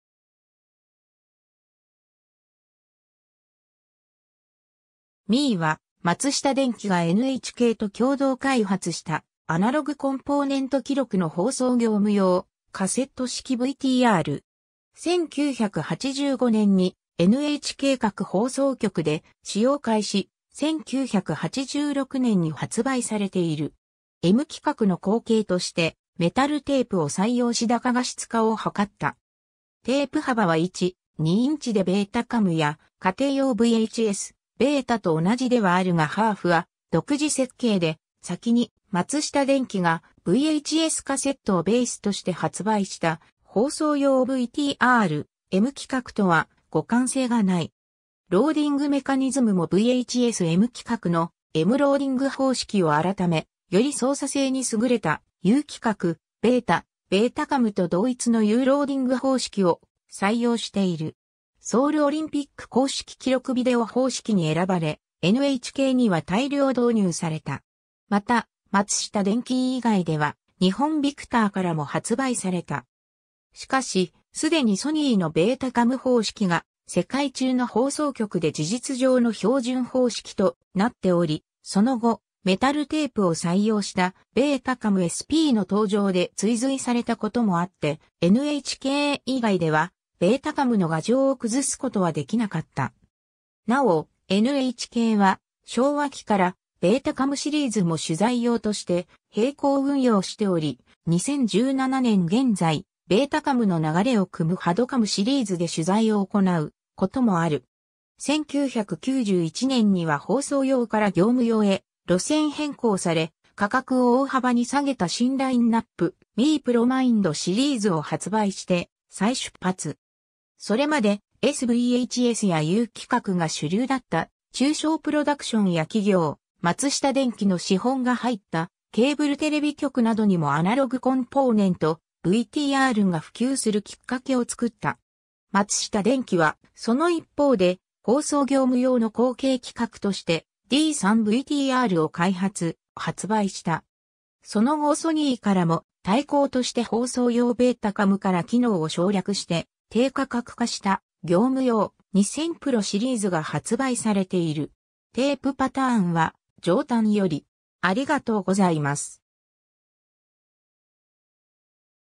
MII
MII（エムツー）は松下電器がNHKと共同開発したアナログコンポーネント記録の放送業務用カセット式VTR。
1985年にNHK各放送局で使用開始、1986年に発売されている。
 M規格の後継として、メタルテープを採用し高画質化を図った。
テープ幅は1/2インチでベータカムや家庭用VHS、Betaと同じではあるがハーフは独自設計で、先に松下電器がVHSカセットをベースとして発売した放送用VTR「M規格」（通称「Mビジョン」）とは互換性がない。
ローディングメカニズムもVHS・M規格のMローディング方式を改め、より操作性に優れたU規格・Beta・ベータカムと同一のUローディング方式を採用している。
 ソウルオリンピック公式記録ビデオ方式に選ばれ、NHKには大量導入された。
また松下電器以外では、日本ビクターからも発売された。
しかし、既にソニーのベータカム方式が世界中の放送局で事実上の標準方式となっており、その後メタルテープを採用したベータカムSPの登場で追随された事もあってNHK以外ではベータカムの牙城を崩すことはできなかった。
尚、NHKは昭和期からベータカムシリーズも取材用として並行運用しており、2017年現在ベータカムの流れを汲むHDCAMシリーズで取材を行うこともある。
 1991年には放送用から業務用へ路線変更され、価格を大幅に下げた新ラインナップ「MIIプロマインド」シリーズを発売して再出発。
それまでS-VHSやU規格が主流だった中小プロダクションや企業、松下電器の資本が入ったケーブルテレビ局などにもアナログコンポーネントVTRが普及するきっかけを作った。
松下電器はその一方で、放送業務用の後継規格としてD3-VTRを開発・発売した。
 その後ソニーからも対抗として放送用ベータカムから機能を省略して低価格化した業務用「2000PRO」シリーズが発売されている。
 テープパターンは上端より、